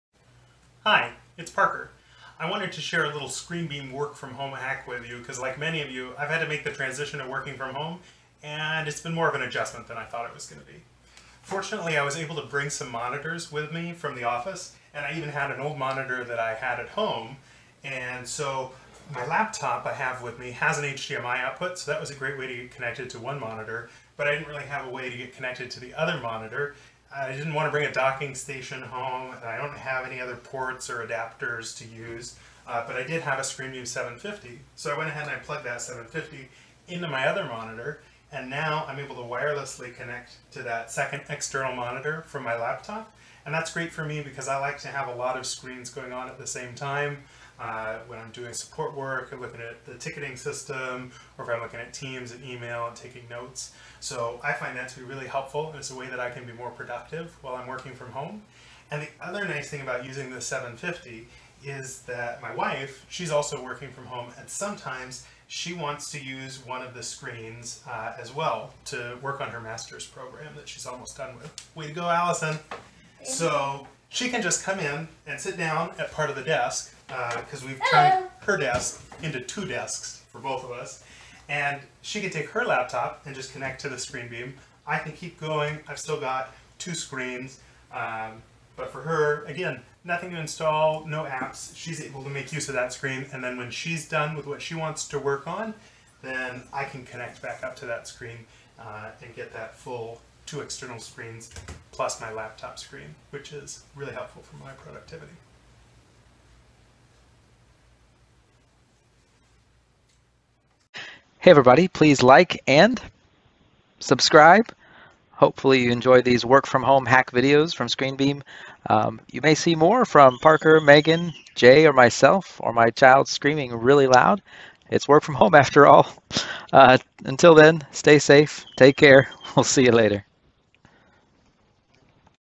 WFH Dual External Monitor Hack with ScreenBeam
This work from home office space hack shows you how to create a shareable, dual-monitor setup without utilizing an expensive, complicated and desk cluttering docking station when all you have is a living room table or folding table to work from.

To hack your own WFH space and to learn more about the ScreenBeam 750
It's easy, no software to install and no network connection required.

More work from home hacks to come!